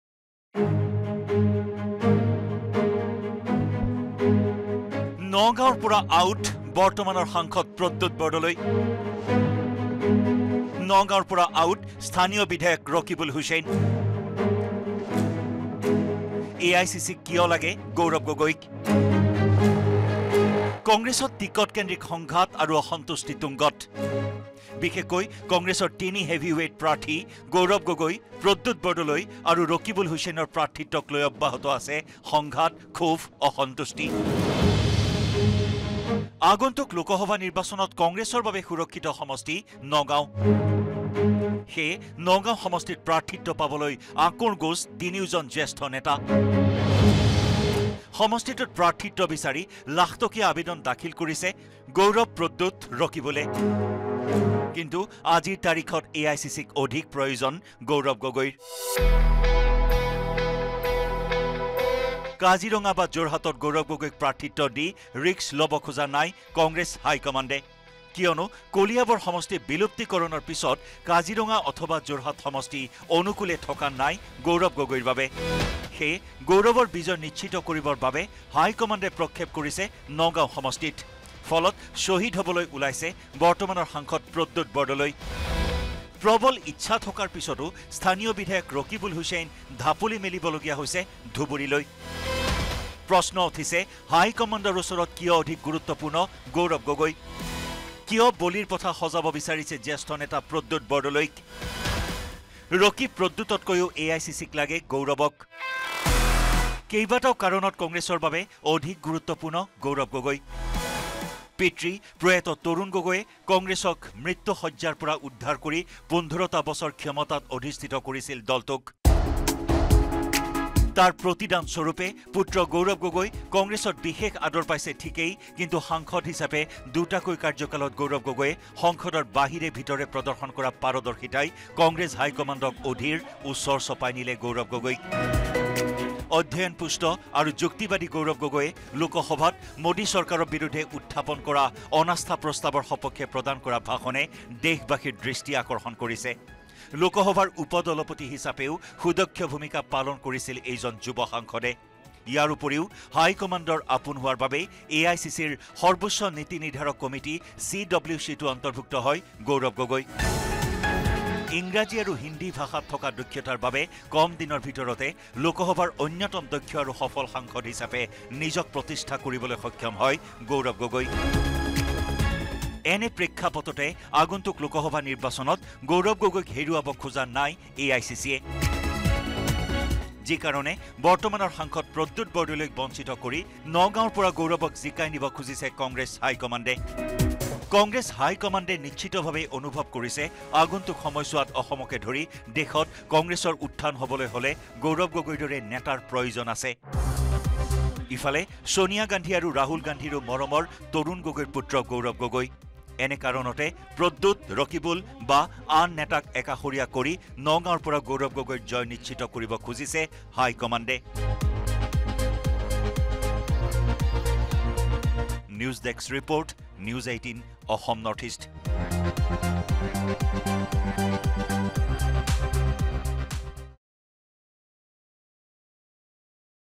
Gaurav Gogoi ৰ বাবেই অথাই সাগৰত প্ৰদ্যুৎ বৰদলৈ আৰু ৰকিবুল হুছেইন ! N18V
নগাঁৱৰ পৰা OUT বৰ্তমানৰ সাংসদ প্ৰদ্যুৎ বৰদলৈ
নগাঁৱৰ পৰা OUT স্থানীয় বিধায়ক ৰকিবুল হুছেইন
AICCক প্ৰয়োজন গৌৰৱ গগৈৰ
গৌৰৱক যোৰহাট বা কাজিৰঙাত দি ৰিক্স ল’ব খোজা নাই হাইকমাণ্ডে
গৌৰৱৰ বাবেই অথাই সাগৰত প্ৰদ্যুৎ বৰদলৈ আৰু ৰকিবুল হুছেইন